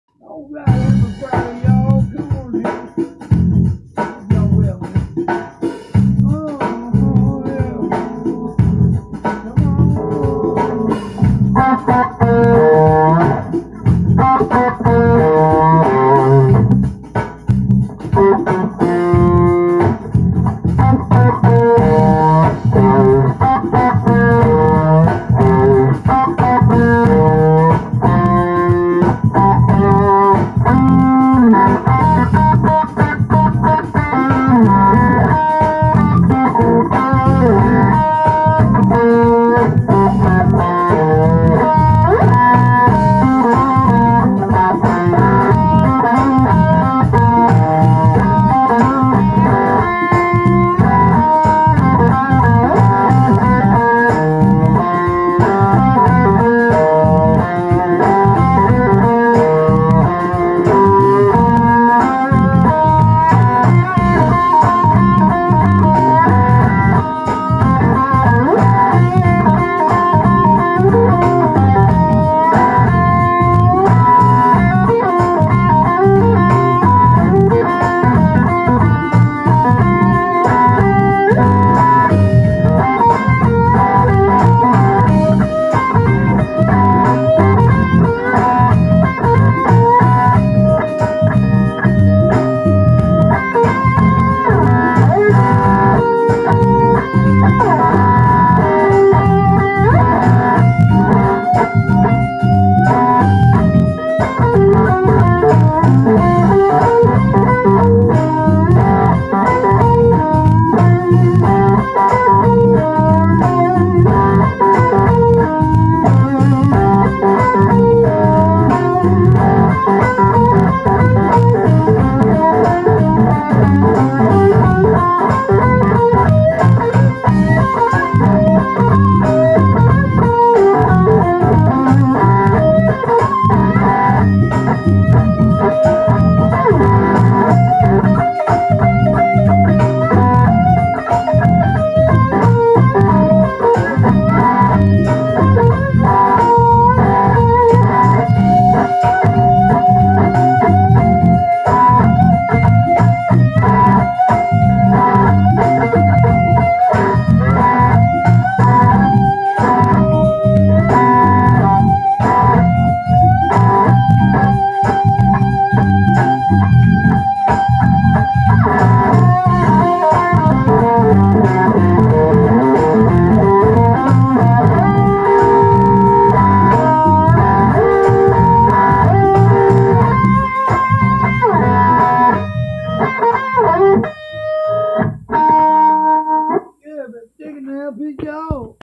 Happy Friday Y'all | Digging This Badass Track in Cm by Petti Music Studios
Bare with me y'all, I'll have my new studio equipment VERY SOON that I'll be using for my daily backing tracks on YouTube, and for my new original music. As always, thank you for your support!